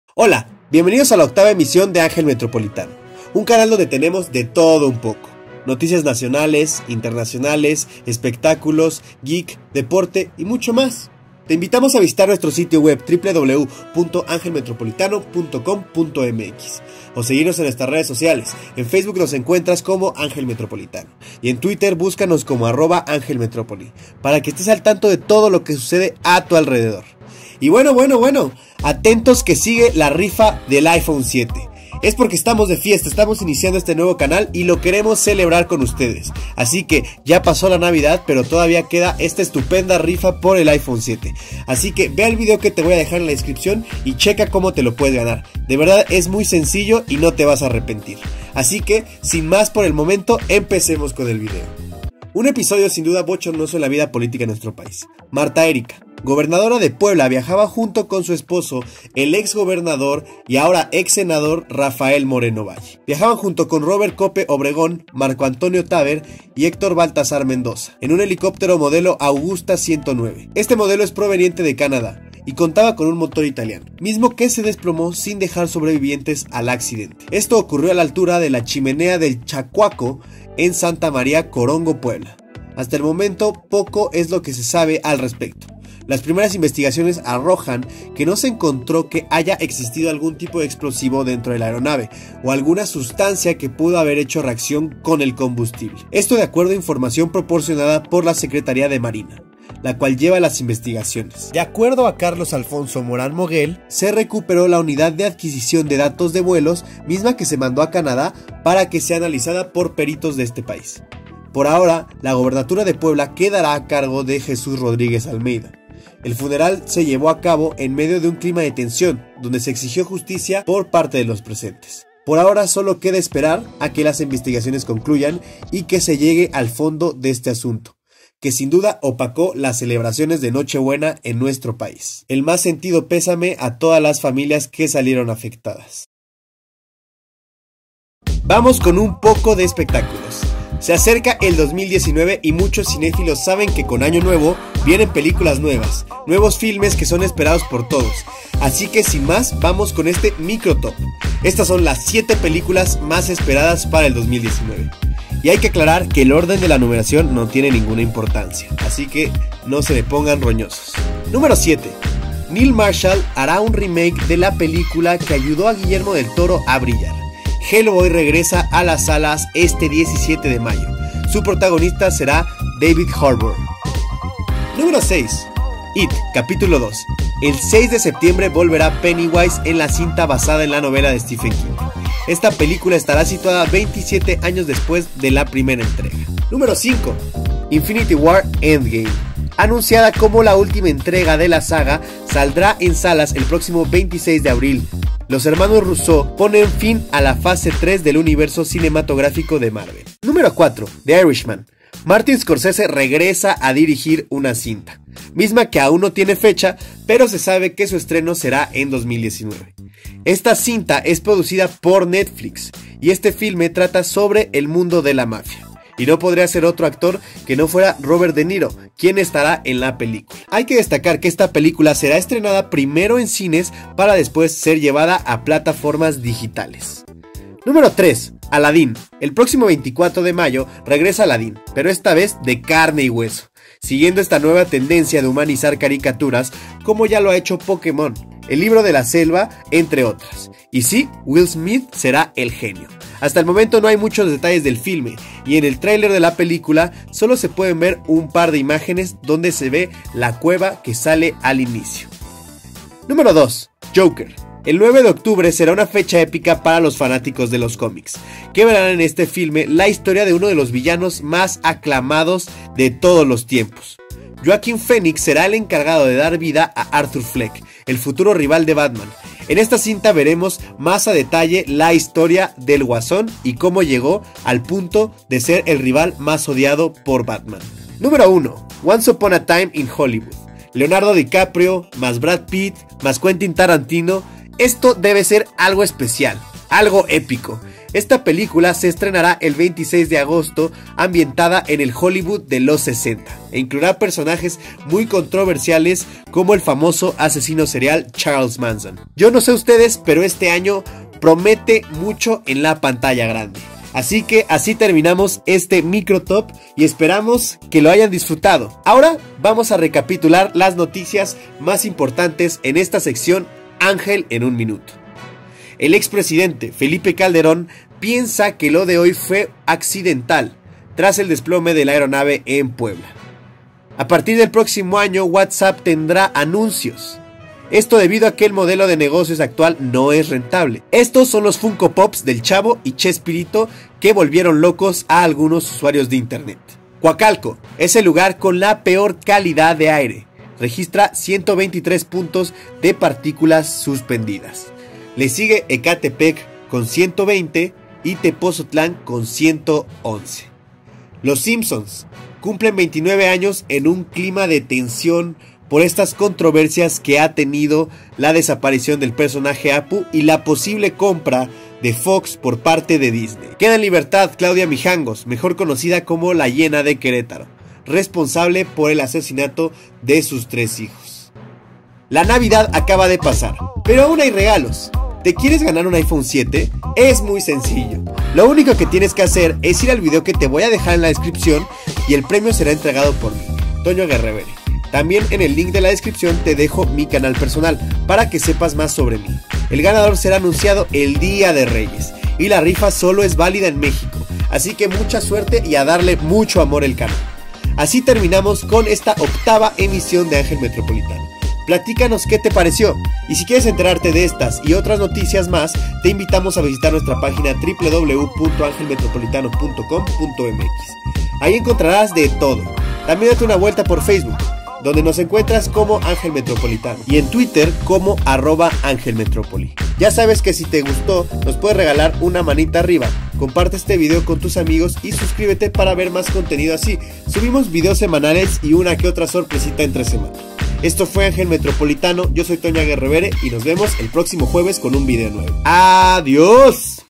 Accidente en Puebla y las películas más esperadas 2019 AM Ep.8😇
Web Erika Alonso, gobernadora de Puebla y Rafael Moreno Valle fallecieron y las 7 película más esperadas del 2019.